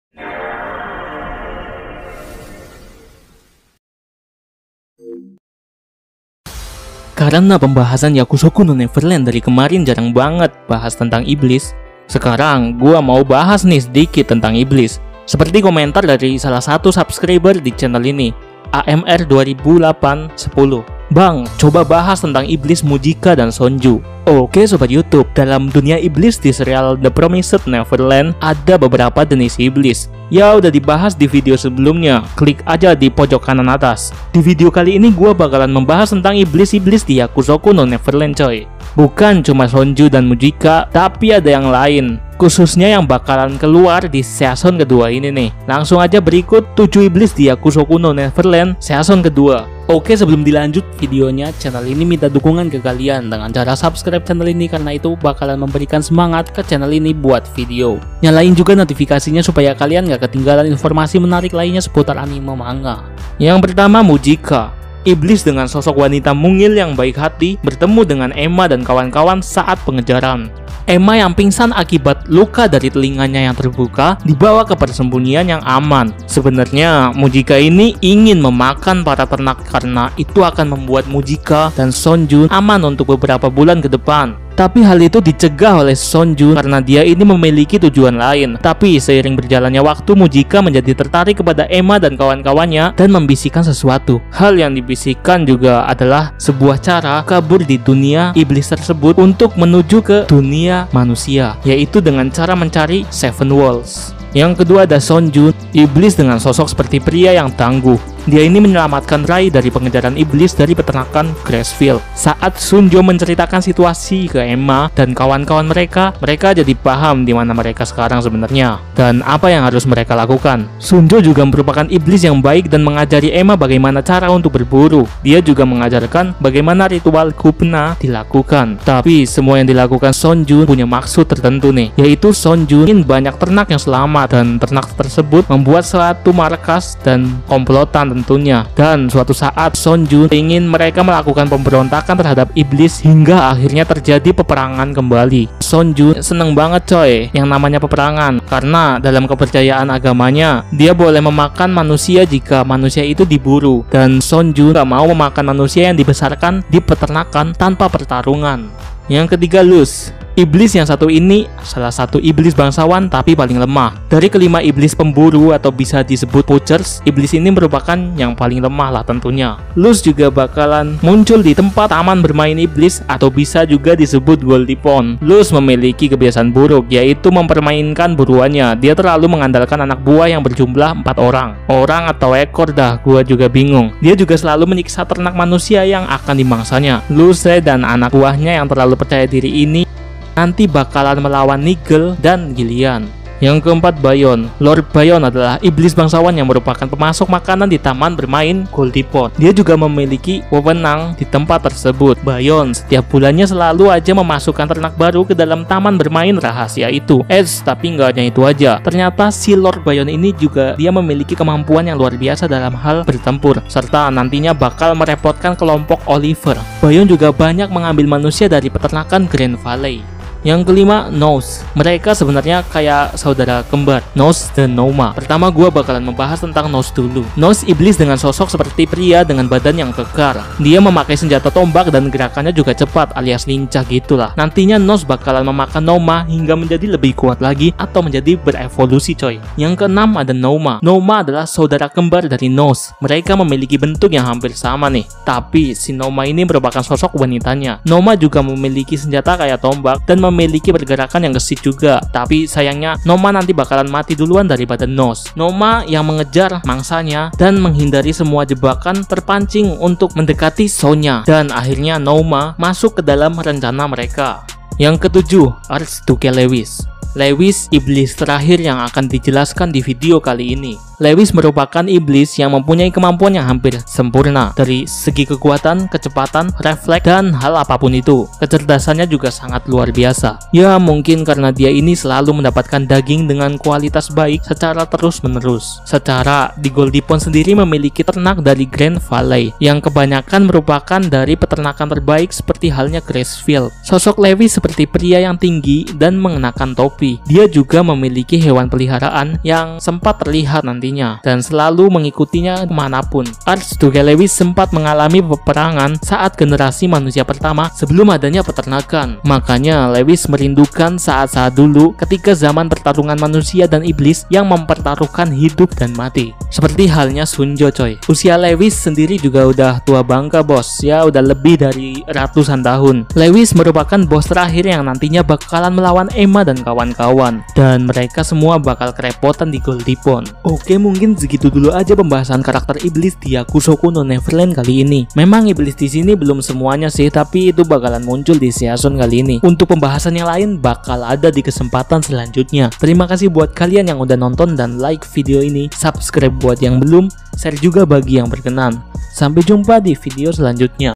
7 KARAKTER IBLIS YAKUSOKU NO NEVERLAND
Karena dalam pembahasan video sebelumnya banyak banget pembahasan yakusoku no neverland dan respon kalian juga baik, kali ini gua bakalan bahas lagi nih khususnya tentang iblis. langsung aja dah simak videonya sampe abis biar ngga gagal paham.

(segala bentuk donasi akan sangat berguna)